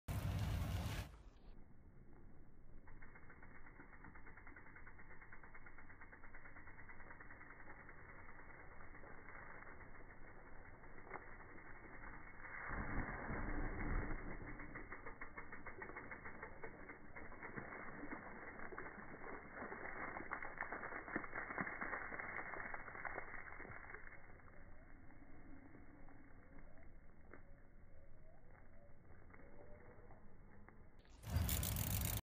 Bmx hydro-plane. Puddle drifting. Big boobs
Puddle drift - slo mo - big boobs - bmx
Chris Spooncer / Sophie Mitchell productions